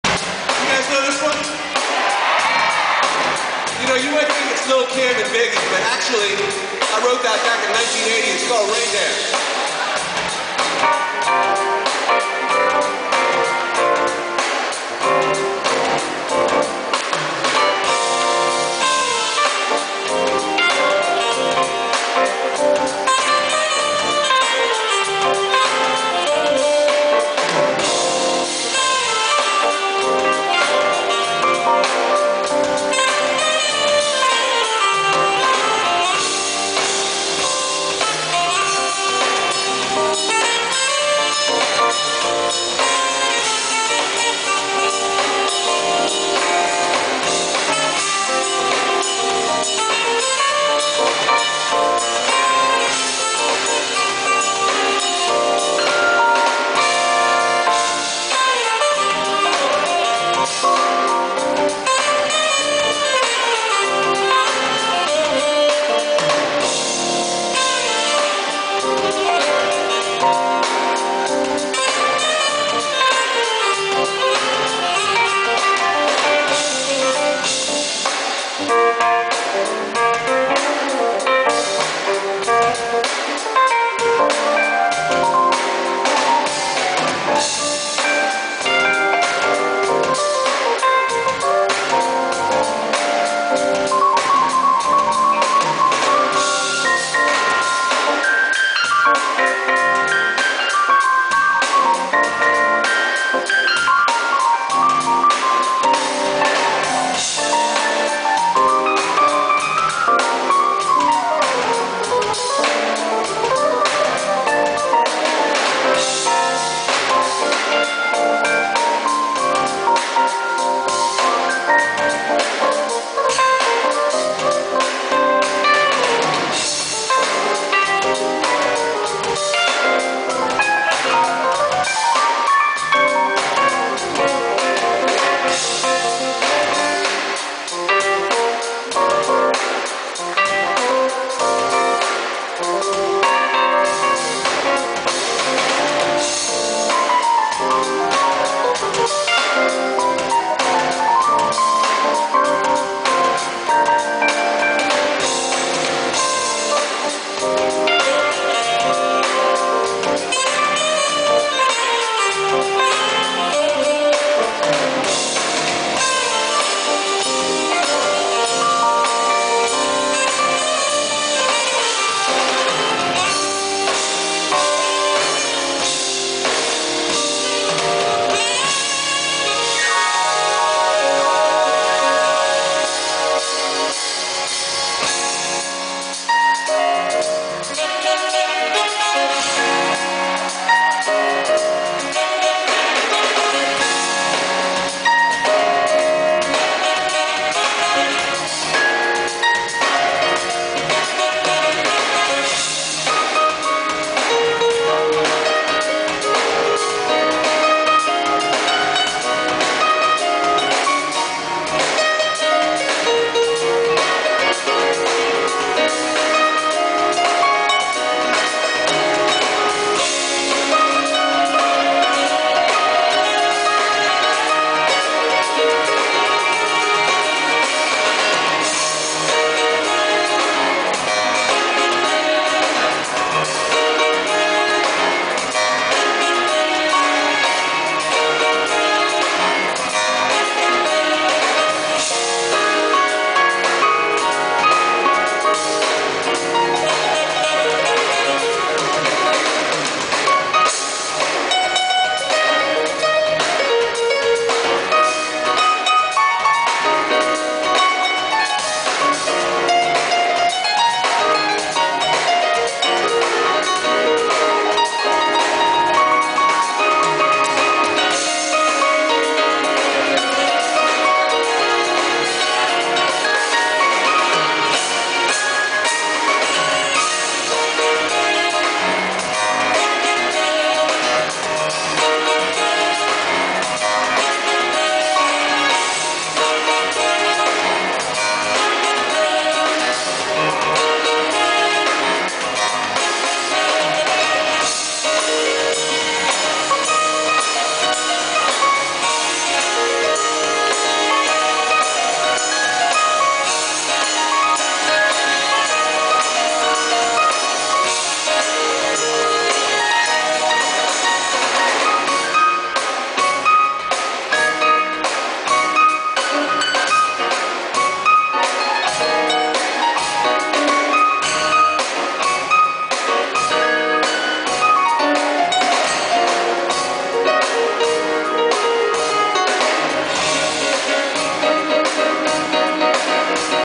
Jeff Lorber@Chicago
Guitar & Saxes...Navy Pier Skyline Stage Chicago.